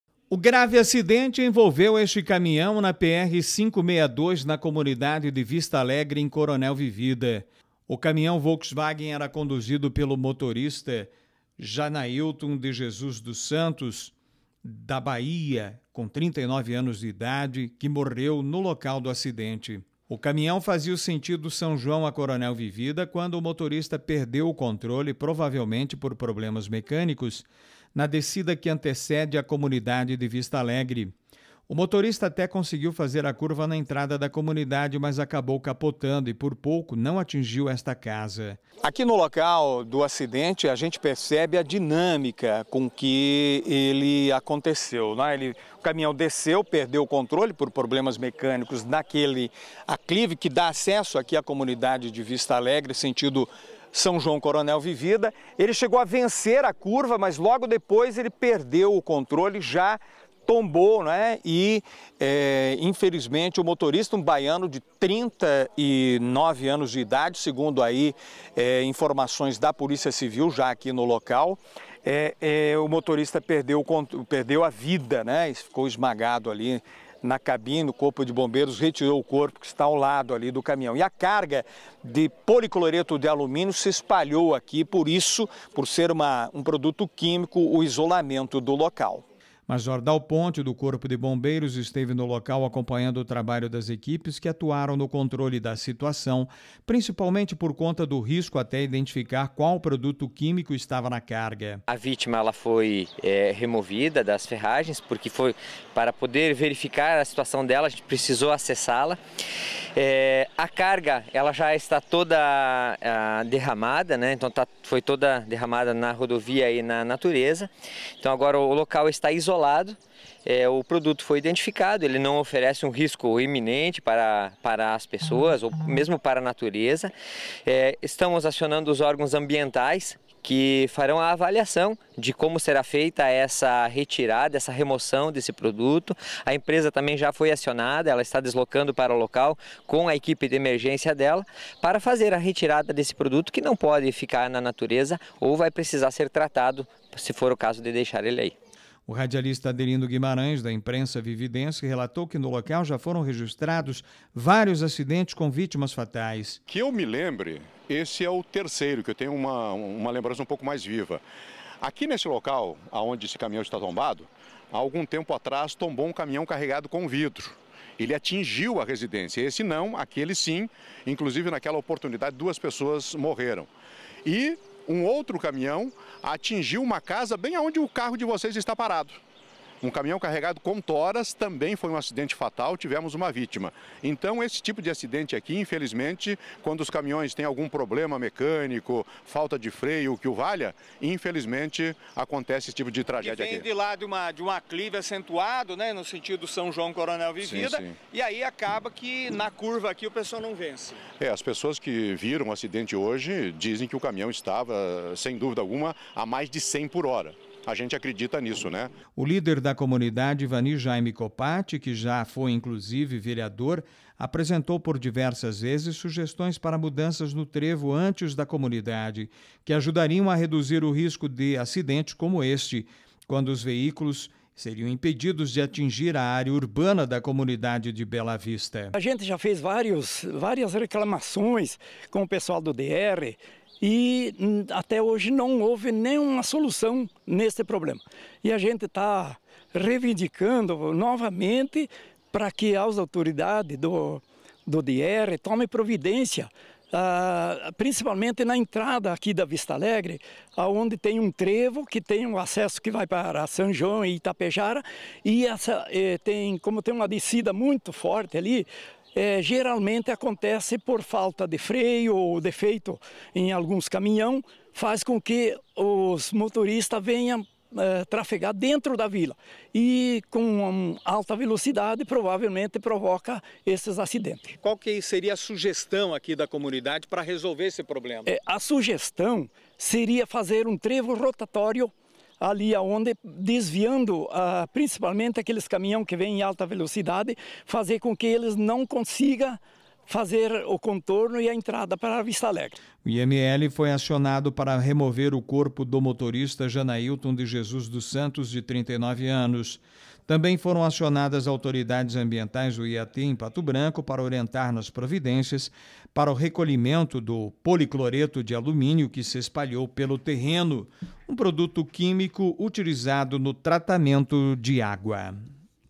Motorista morre em grave acidente na Comunidade de Vista Alegre em Coronel Vivida
Segurança: O grave acidente envolveu um caminhão na PR-562 na Comunidade de Vista Alegre em Coronel Vivida. O motorista morreu no local.